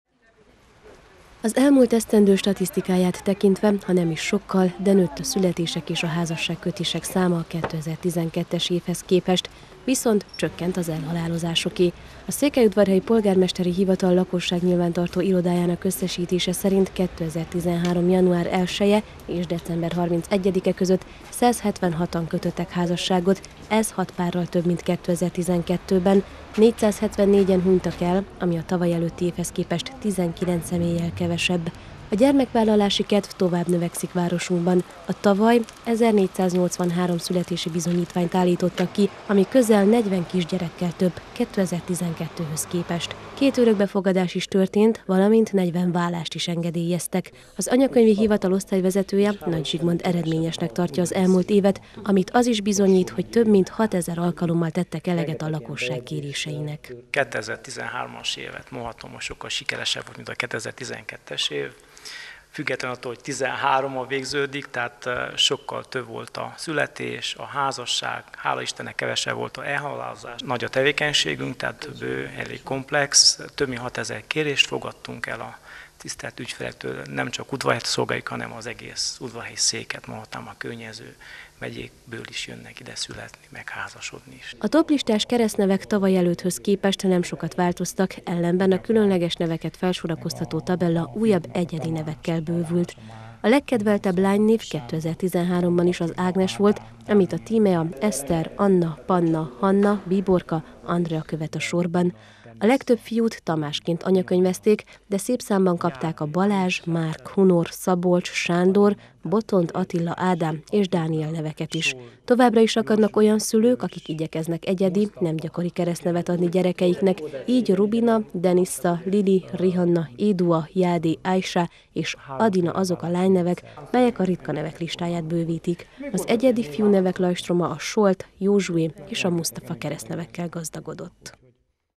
Nőtt a gyerekvállalási kedv 2013-ban
Az elmúlt év statisztikáit tekintve jó hír, hogy Székelyudvarhelyen nőtt a születések száma és csökkent az elhalálozásoké. A Polgármesteri Hivatal lakosságnyilvántartó irodájának összesítése alapján 2013-ban, ha nem is sokkal, de a házassági kedv is növekedett városunkban. A keresztnevek tekintetében nem sokat változott a trend. Tavaly is az Ágnes és a Tamás bizonyult a legnépszerűbbnek, de Moustafát és Rihannát is anyakönyveztek.